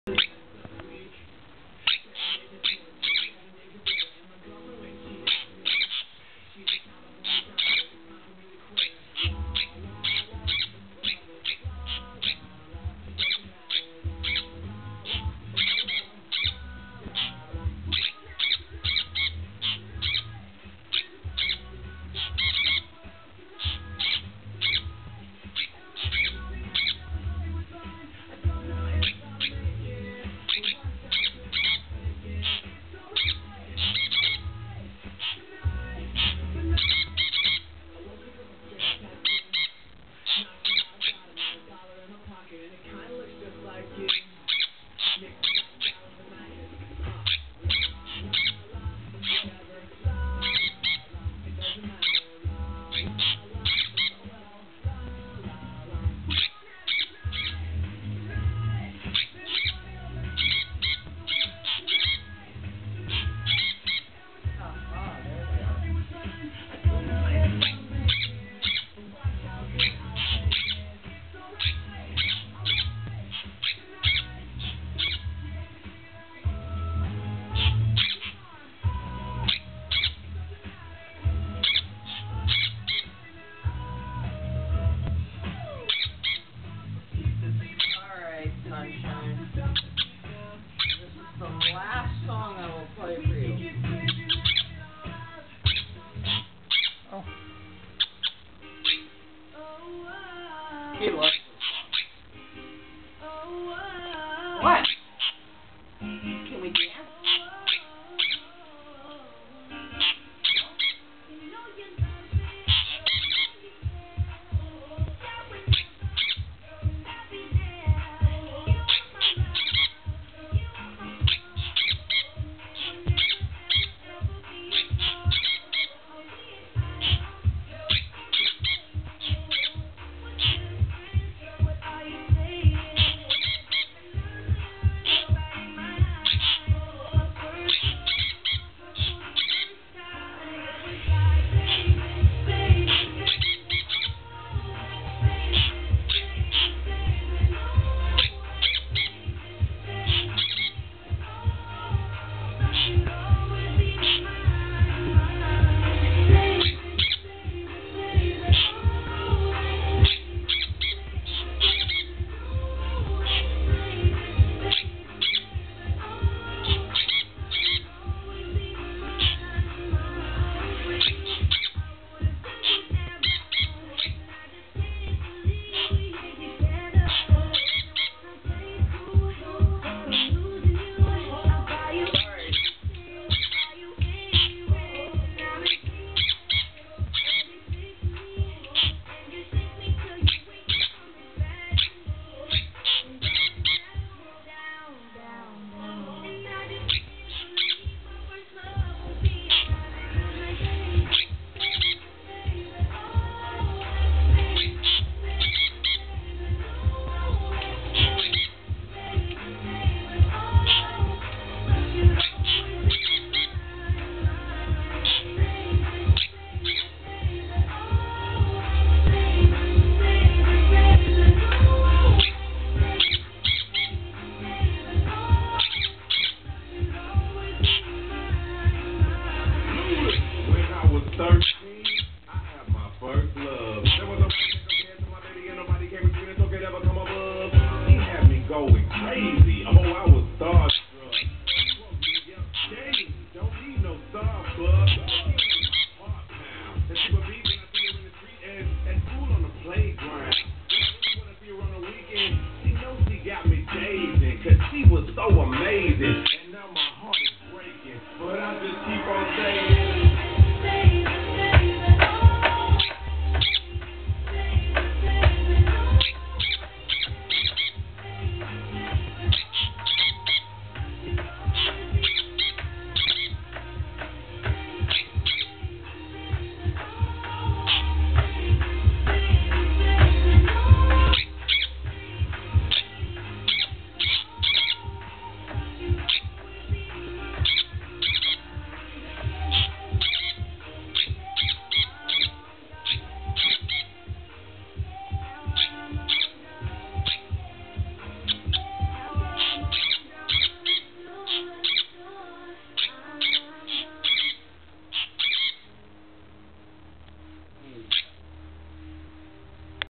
Sunshine the Sun Conure Singing & Dancing
Sunshine Loves to Sing and Dance